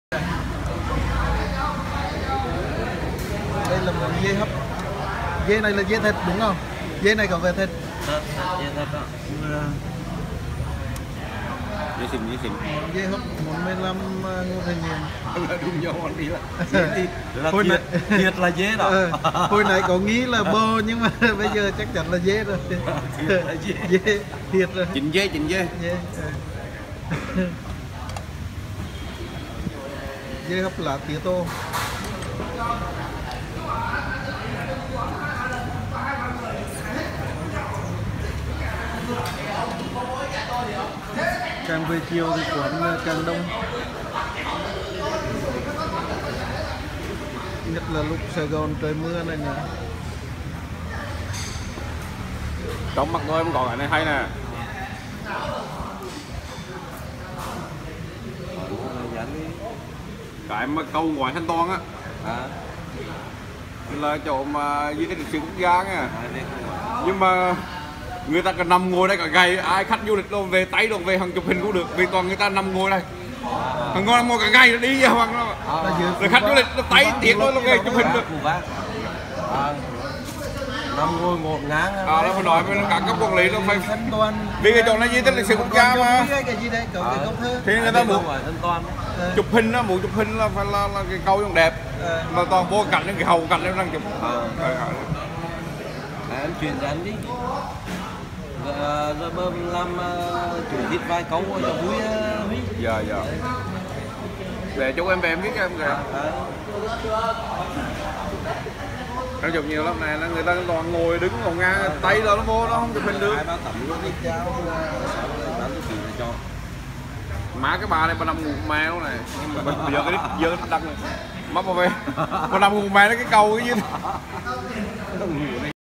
Dê hấp ngon nhức răng ở Sài Gòn
Dê 45 Ngô Thời Nhiệm quận 3. Dê hấp tía tô.